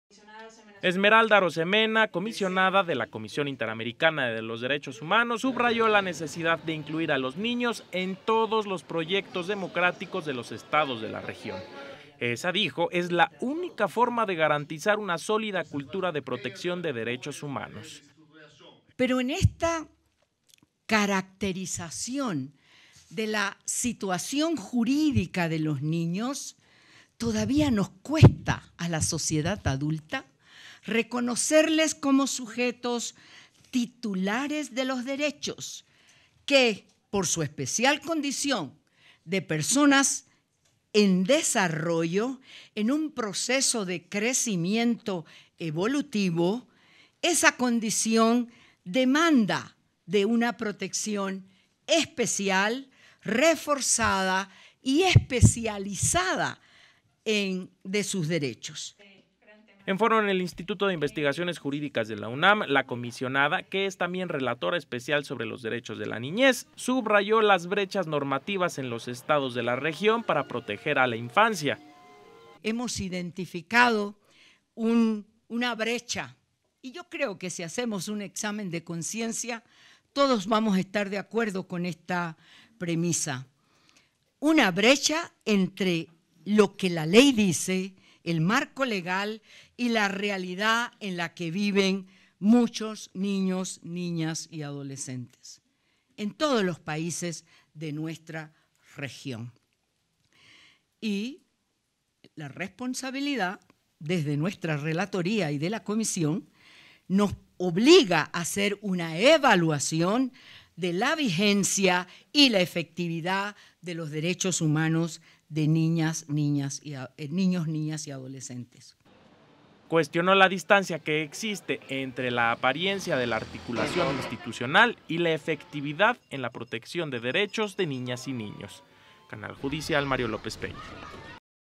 Recomiendan reconocer a los niños como sujetos de derechos.
De acuerdo con académicos, estos 5 mil programas aplicados en el país, no han tenido un verdadero enfoque de ayuda para la población que más lo necesita.